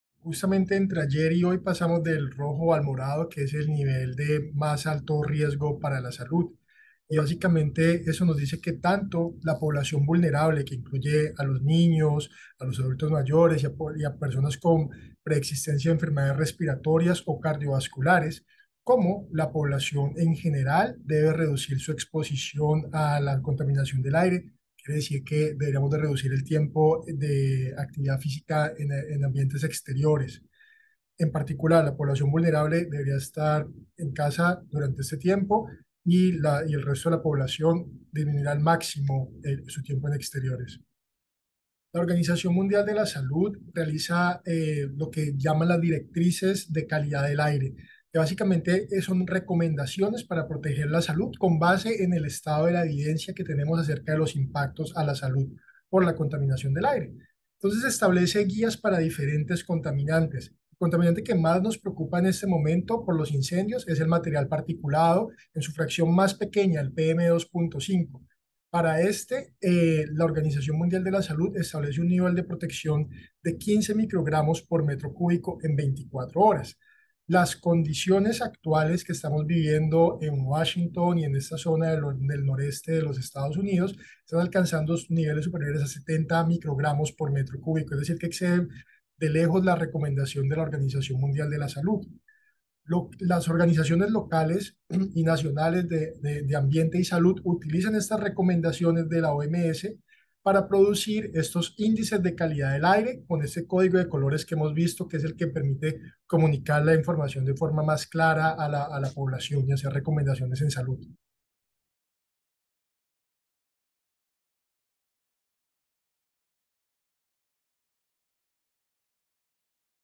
Contaminación por incendios -Juan Castillo- especialista OPS.
El asesor regional en calidad del aire y salud de la Organización Panamericana de la Salud (OPS) Juan José Castillo Lugo en entrevista con la Voz de América explica el impacto de la contaminación ambiental causada por incendios y otros factores en en EEUU y la región latinoamericana.